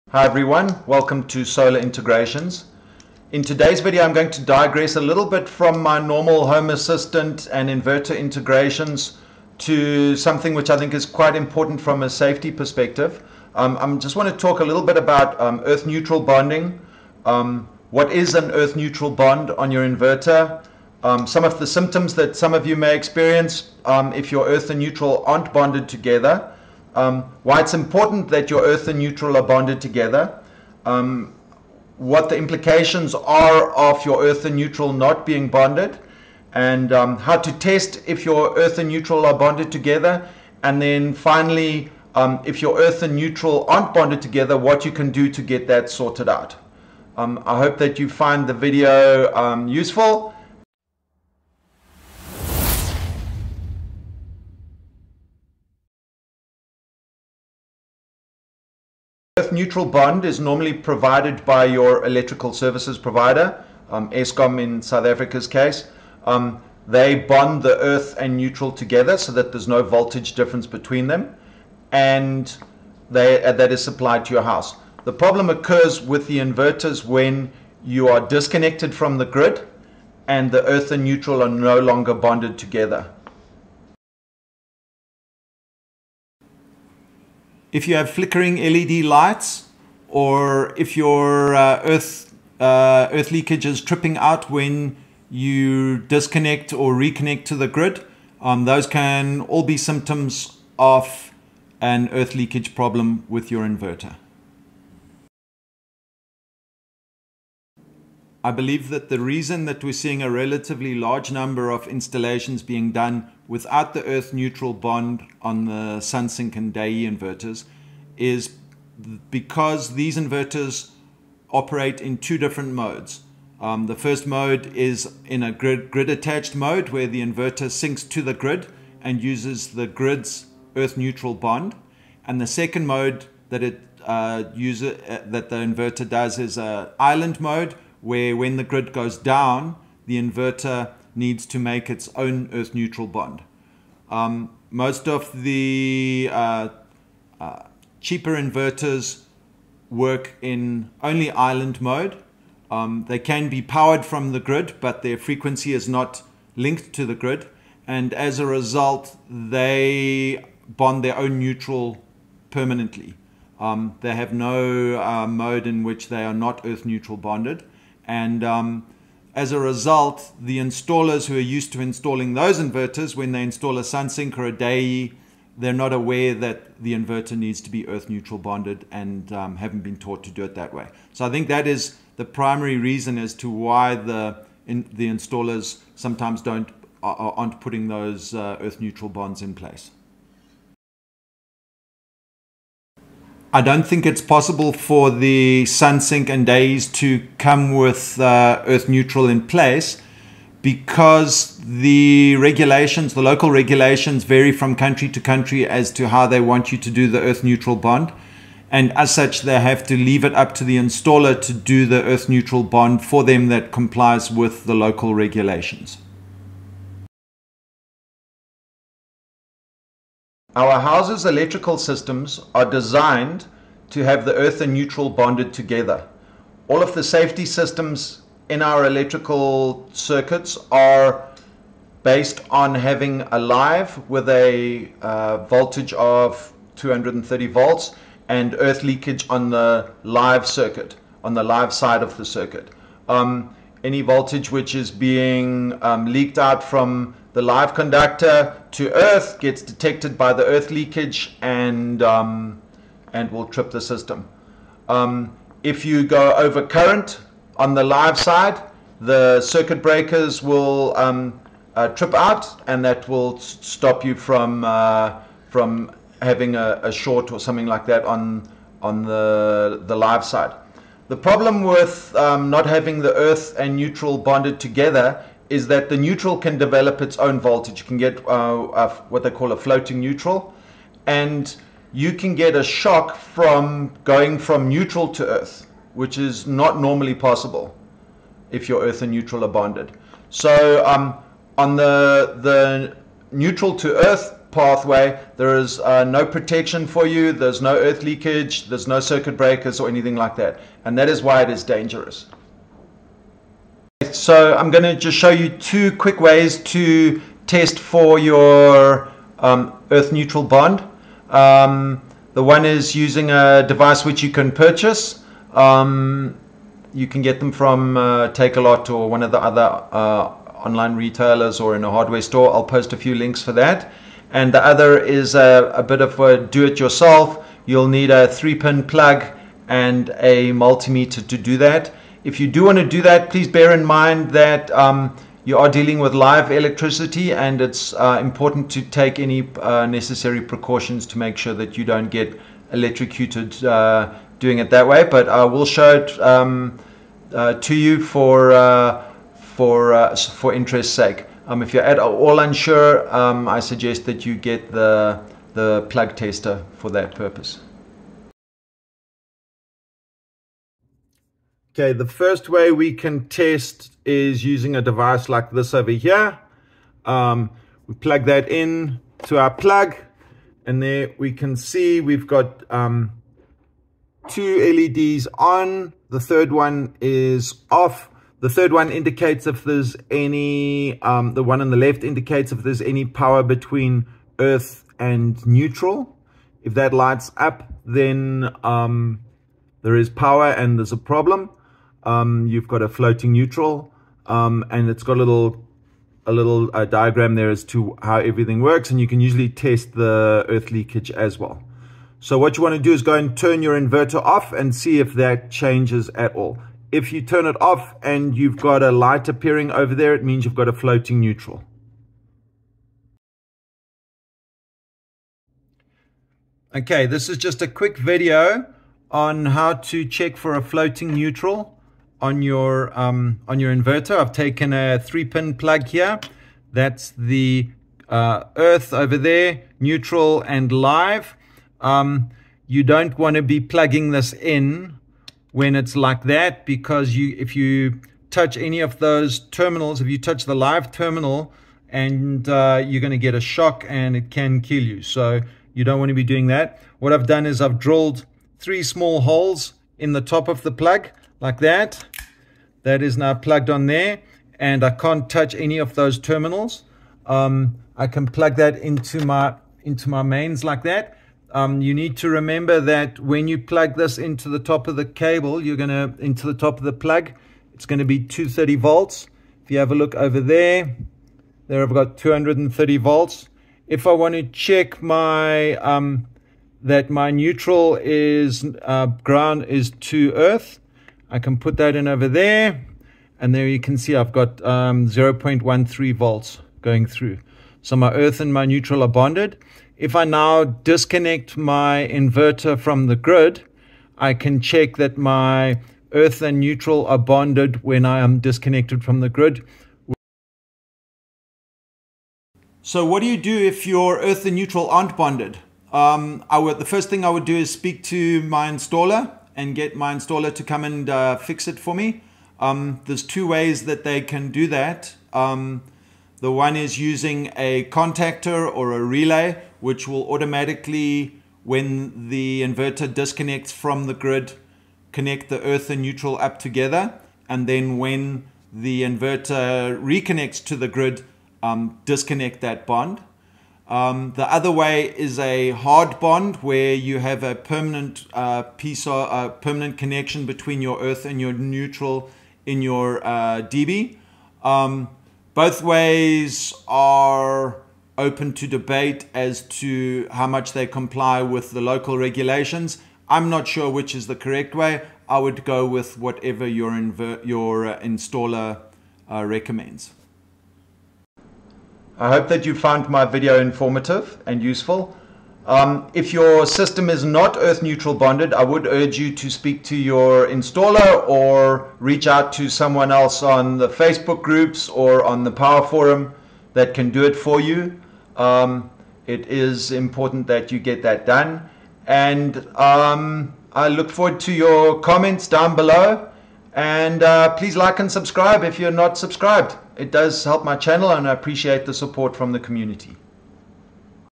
The first thing you should check on your new Inverter installation - Earth/Neutral Bonding
I decided to make this video due to the large number of unbonded solar installations I have seen people asking questions about on the Facebook groups and the thought it would be a good idea to produce a layman's guide to Earth Bonding, what it is and why it is important that you check that your inverter has an Earth/Neutral bond enabled.

Please remember that I am a hobbyist solar enthusiast and not a qualified electrician or solar installer.  I have attempted to make this video as simple to understand as possible, my terminology may not all be 100% correct, if it isn't, please forgive me and let me know in the comments section so that I can correct it.

Unbonded systems are inherently dangerous and need to be made safe as soon as possible by a qualified installer.

Plans to build your own socket tester on instructables